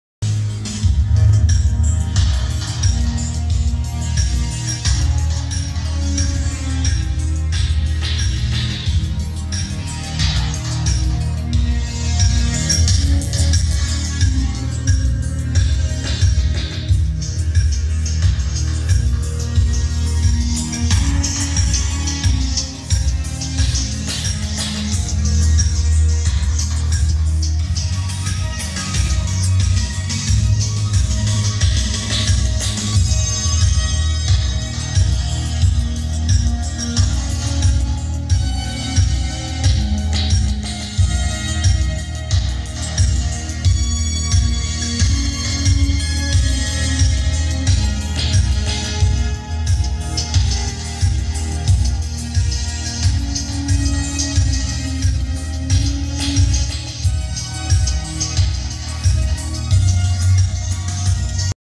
Tonsil Alton 70 + Onkyo A-8420
o to Polskie kolumny którym żaden chiński chłam nie dorówna żadne creative logitechy i wieże chińskie są to kolumny polskiej produkcji i niby moc 70 watt jedna a gra dużo lepiej i głośniej od tych gdzie piszę 10000000 watt   i jest to moc prawdziwa a nie wźięta z kosmosu zapraszam do komentowania